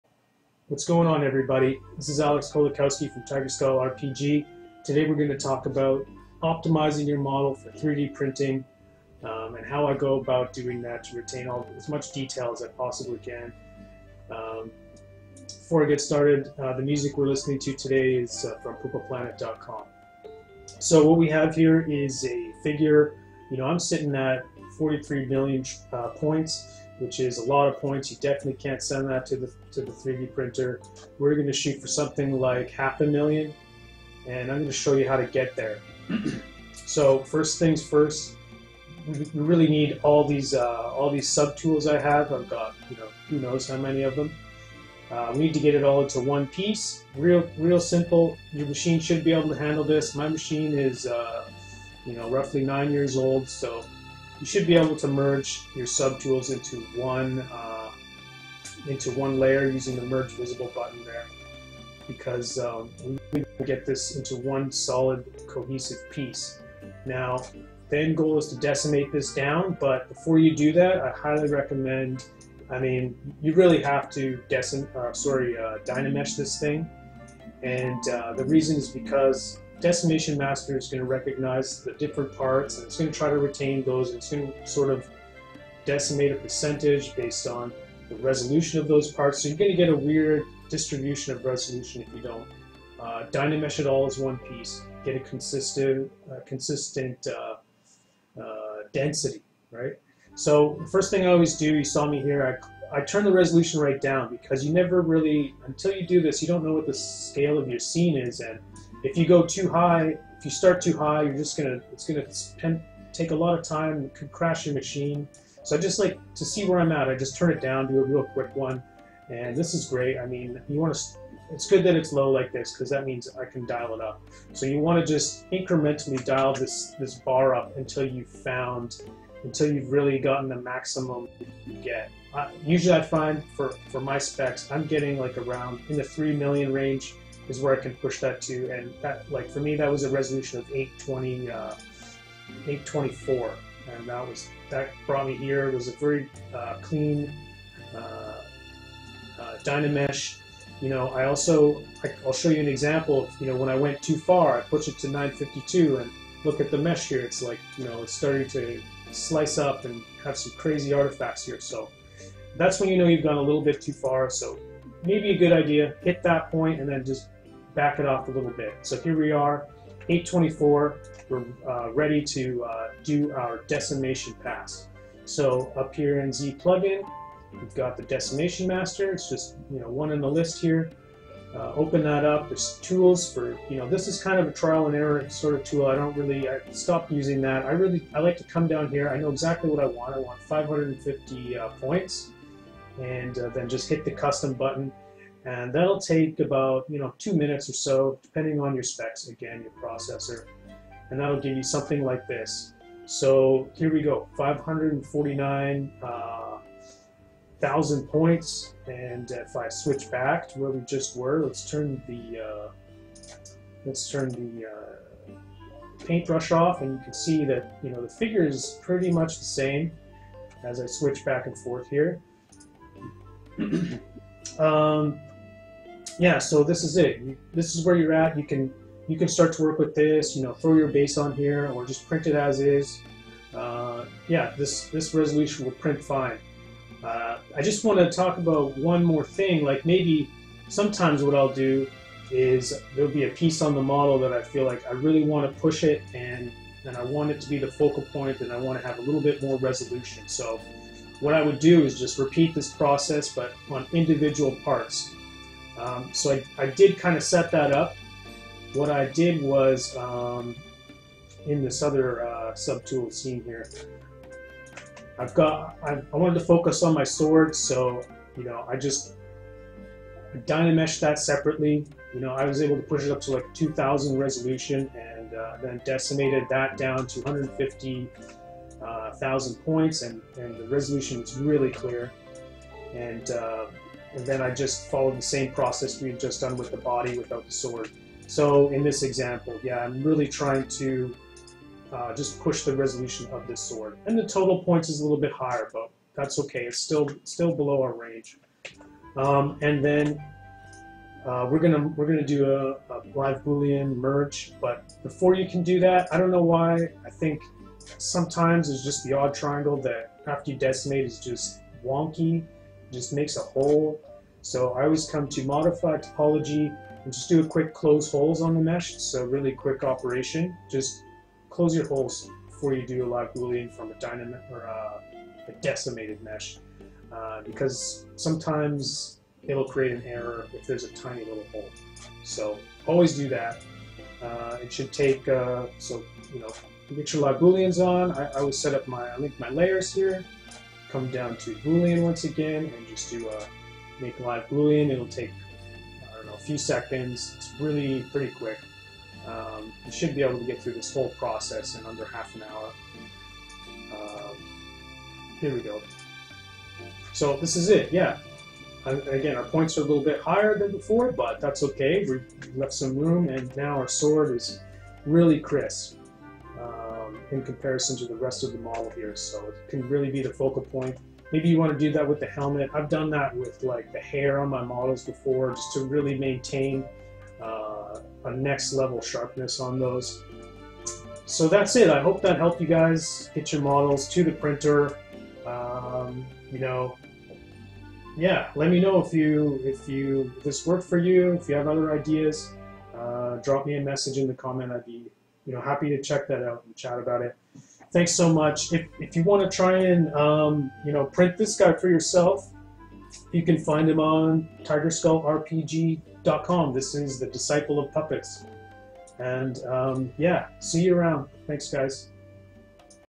How to optimize your model for 3D printing
In this tutorial I describe my process for taking a finished, high resolution sculpt to the necessary resolution for 3d printing. I describe how to do this with minimal detail loss and discuss strategies to improve quality in specific areas as desired. This is intended for zbrush users of all skill levels as every command is shown in the default zbrush Ui.

If you're interested in printing this model for yourself, head over to
to find all of my tabletop RPG collections.

Tablet I use